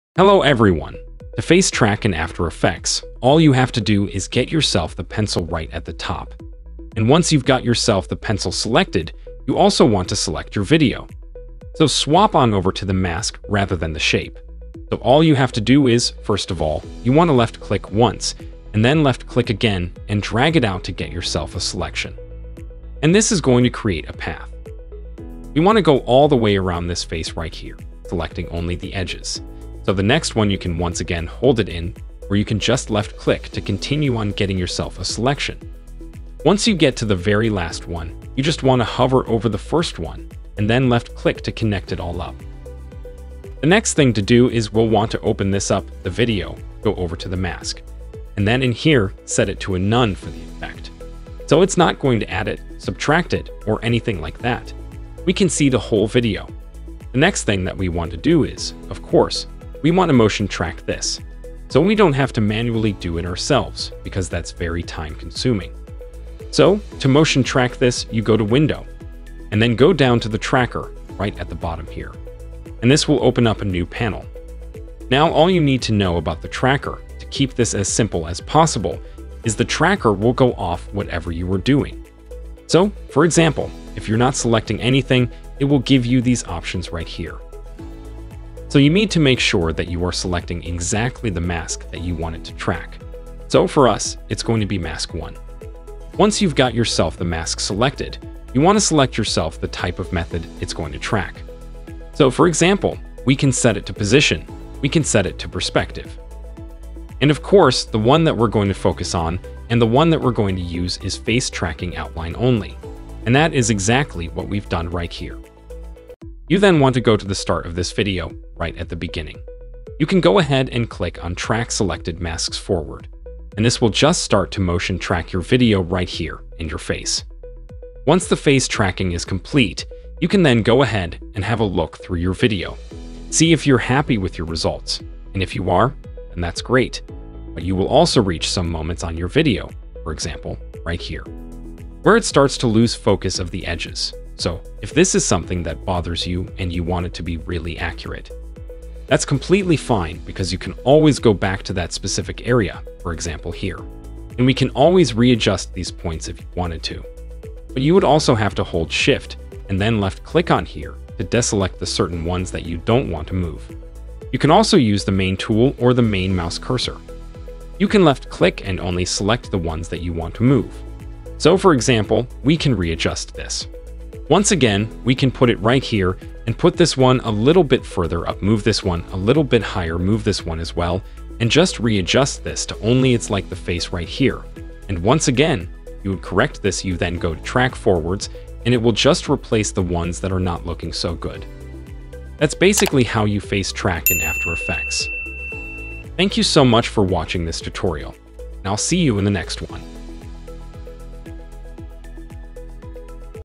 How To Motion Track Masks in After Effects 2023 (Face Tracking)
Learn the latest techniques for Motion Tracking Masks in After Effects 2023, with a special focus on Face Tracking. This comprehensive tutorial will guide you step-by-step through the process of creating seamless and professional motion-tracked masks for your videos. Whether you're a beginner or looking to refine your skills, this tutorial has you covered. Dive into the world of motion graphics and special effects with After Effects 2023!